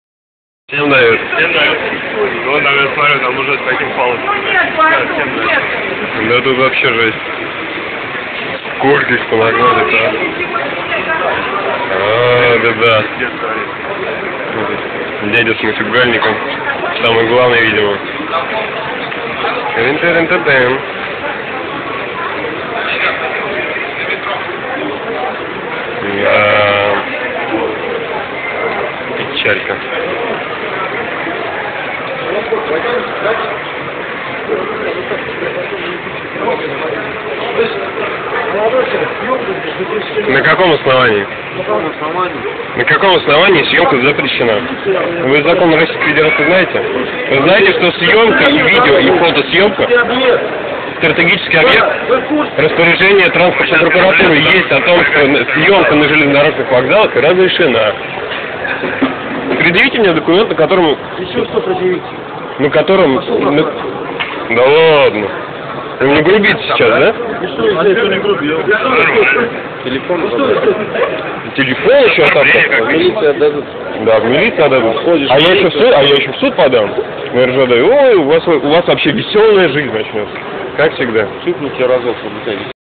Облава на Курском вокзале 8.06.11. Общение с "ловцами".
Снимая весь тот цирк, который сегодня творился утром на Курском, получил вполне ожидаемую реакцию со стороны т.н. охраны. В лучших традициях быдла из провинции, наделенного крупицей власти.